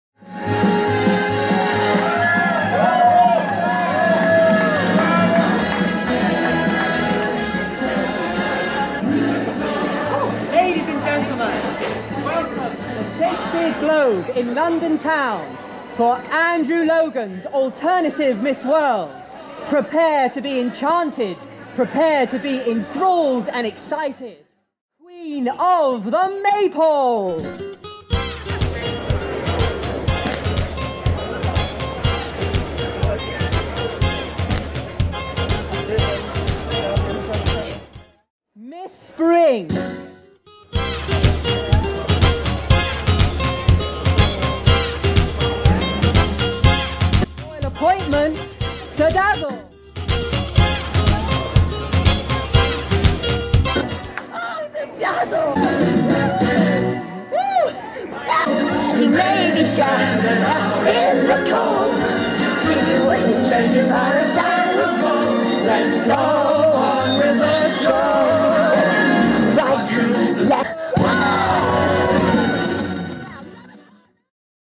Mister Show Time & Donna Maria's Maypole at Shakespeare’s Globe
Mister Show Time and Sir Dazzle join the fabulous Miss Maypole and Miss Spring for the appearance of Donna Maria's Maypole at Andrew Logan's Alternative Miss World, at the Shakespeare's Globe